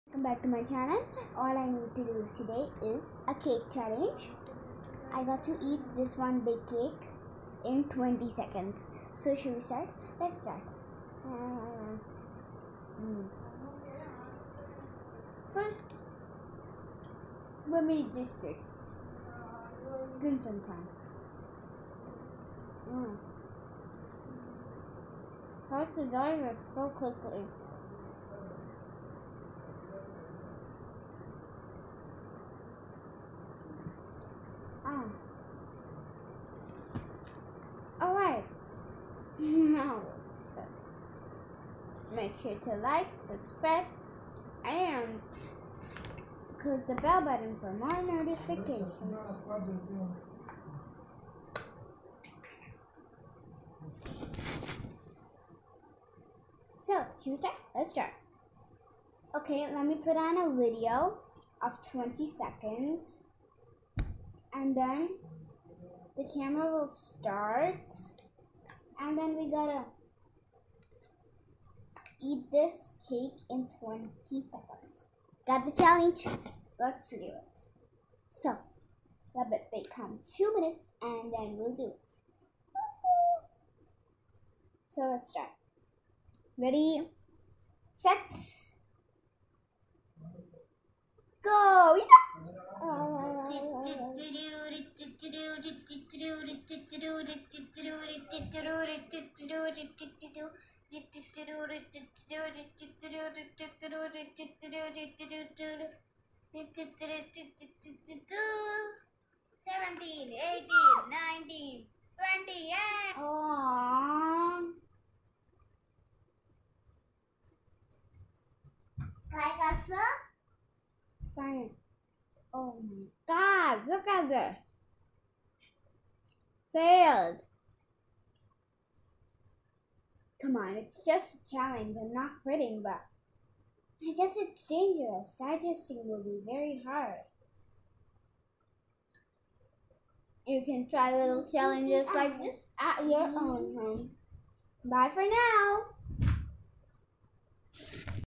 5 June 2021
cake challenge
for fun just
pretend Play
by Ammu and Pandu little sisters
funny fail.kid fun challenges
Amuktha's videos by (. )private number for kids security